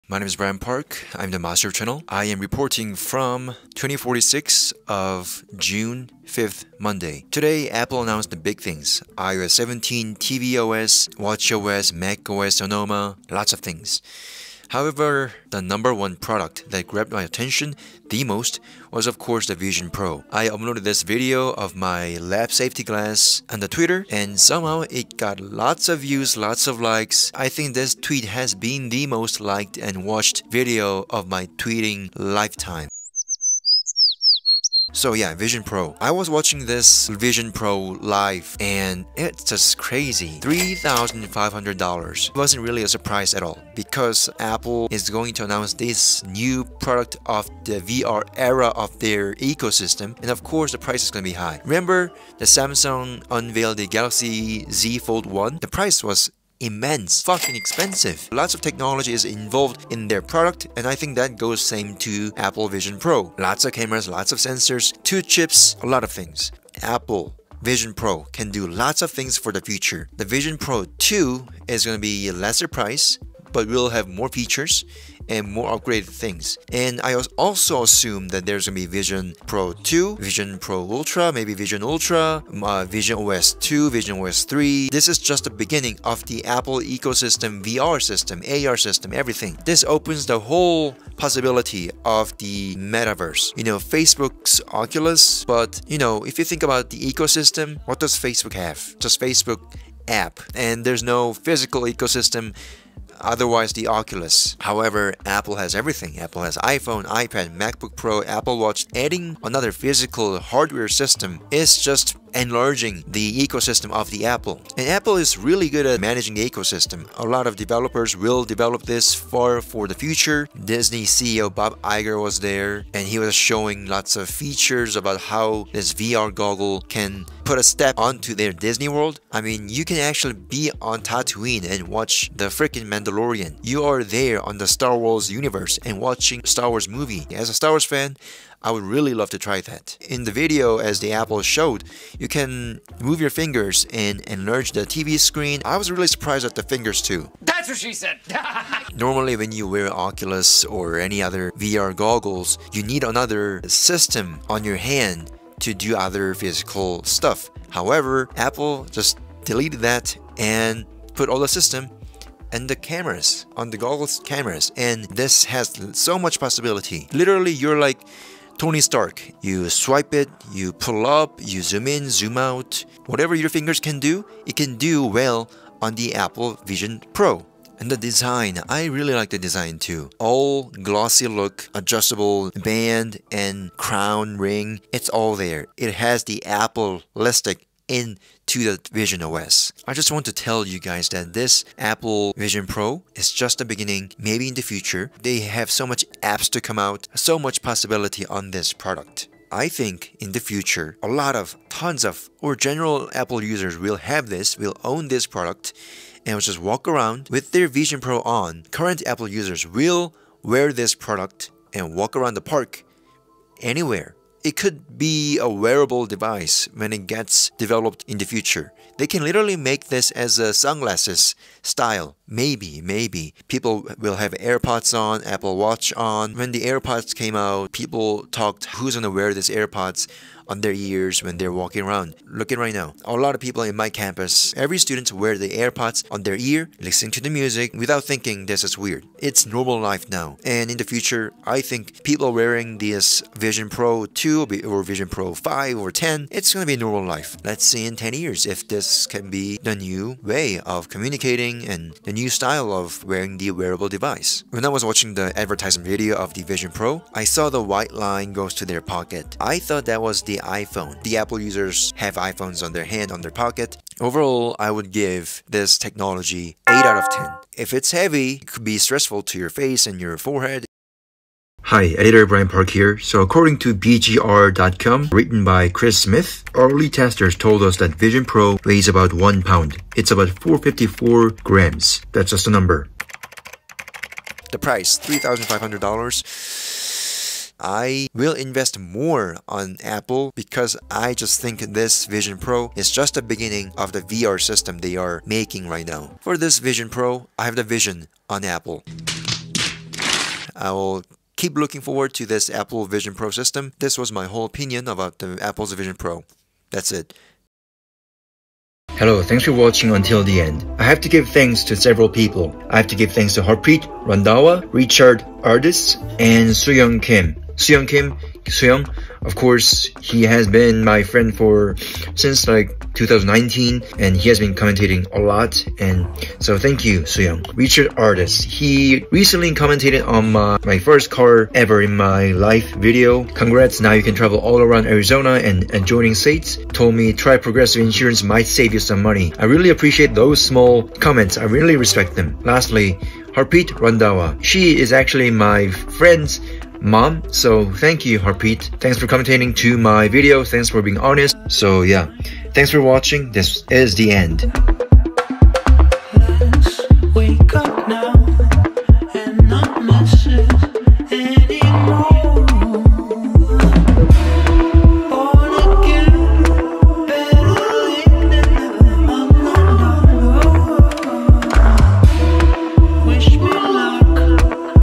My thoughts on Apple's Vision Pro;
This video is filmed on the same day as Apple's WWDC 2023 (June 5th, 2023). Do you think Apple is making a BIG STEP FORWARD on VR and AR world?

BIG shoutout to @harpreetrandhawa7288 & @richardardis1405

Gears I used for the video:
📍Computer: MacBook Pro 16inch 2021 M1 Pro
📍Camera: iPhone 14 Pro
📍Sound: DJI Wireless
📍Editing: Final Cut Pro X